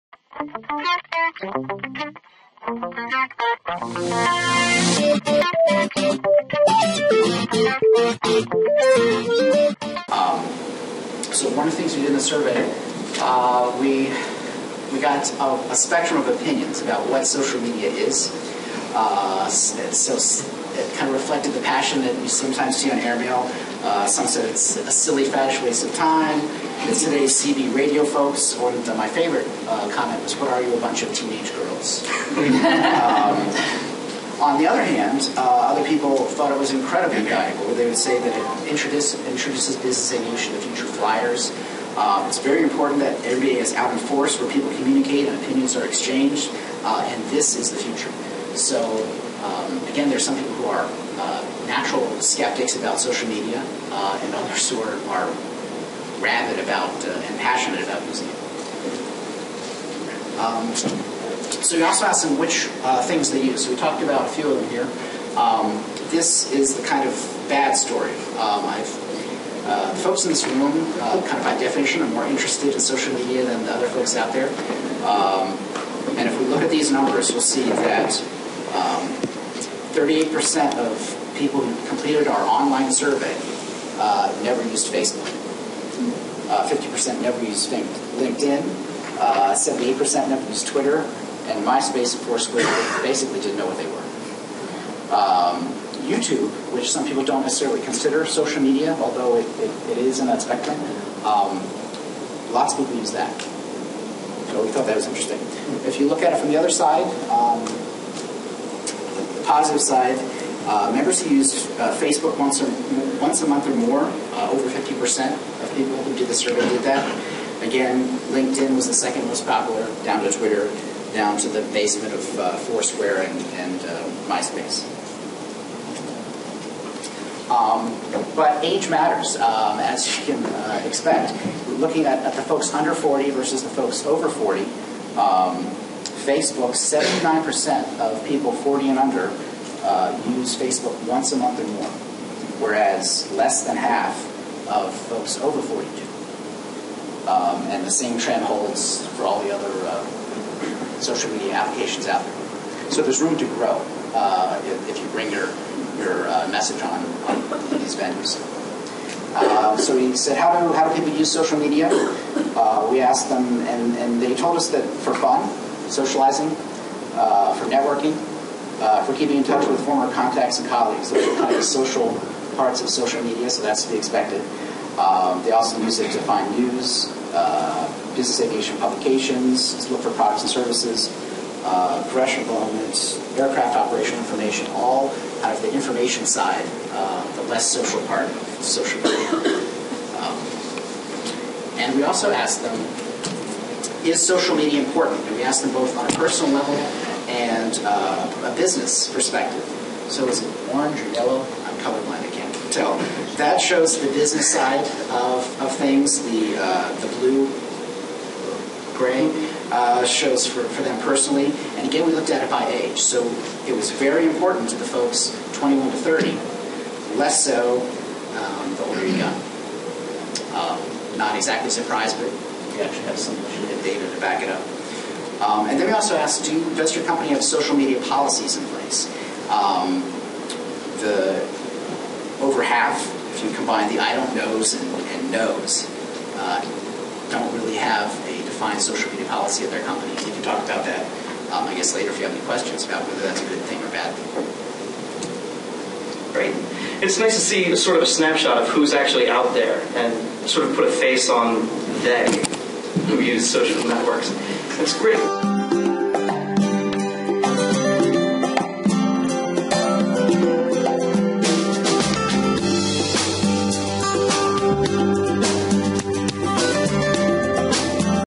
Who in Aviation Uses Social Media? NBAA Member Survey results
At the Social Media Panel at NBAA10 in Atlanta - See the whole panel discussion